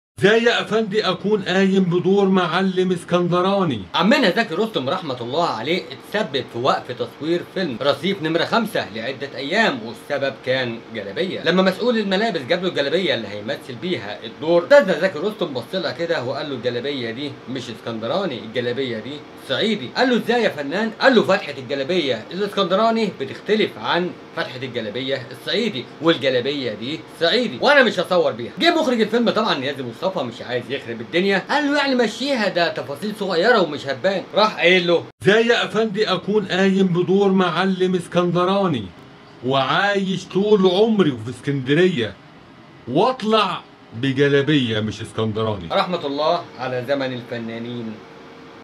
زكي رستم يمتنع عن تصوير مشاهد رصيف نمرة 5
الفنان زكي رستم يمتنع عن تصوير احدي مشاهد فيلم رصيف نمرة 5
وذلك بسبب جلابيه فقد جاء له متخصص الملابس بجلابية صعيدي فرفض التصوير بها لانها تختلف من الرقبه عن الجلابية الاسكندراني .
فيديو تم انشاؤة بالذكاء الاصطناعي واخذ الصوت من شريف ياسين وقان بالتعديل عليه ثم قمت بالتعديل علي الصور بالذكاء الاصطناعي.